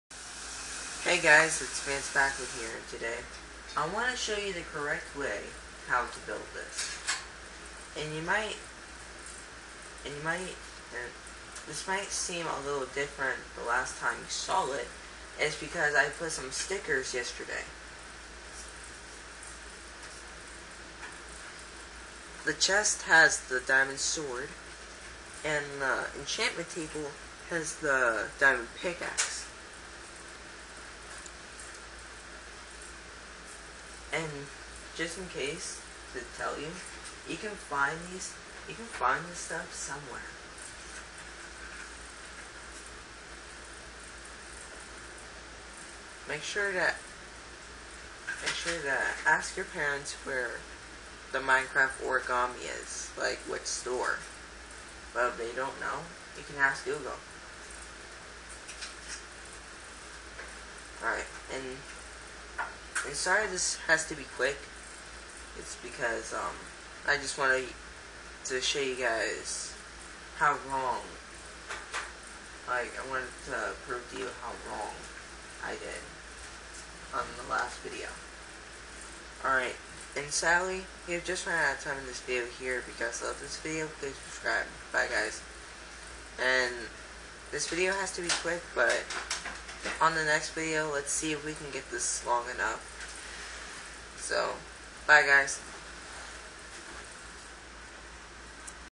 Showing the correct way to build the minecraft origami
This is the correct way how to make the stuff from minecraft and don't forget to add me on Instagram, my name is vancepackwood.
Vance packwood question (Last question until further notice) : Do you like origami?